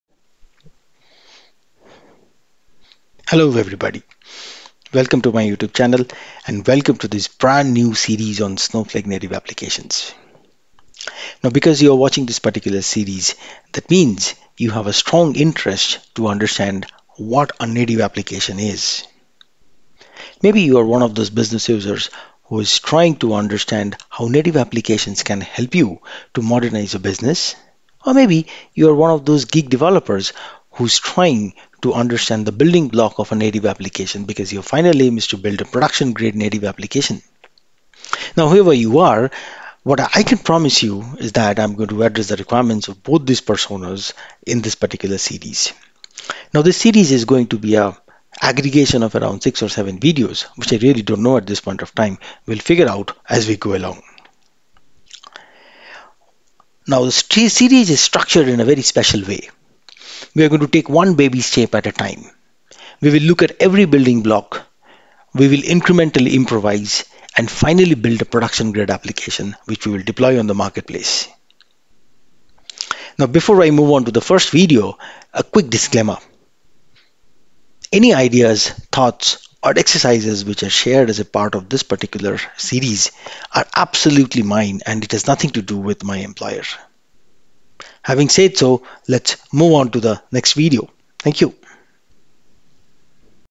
Introduction To Snowflake Native Application Developer Series
Welcome to a brand new series on Snowflake Native Application Framework !!
This series has been designed for you to learn and build production grade Native Applications from the ground up.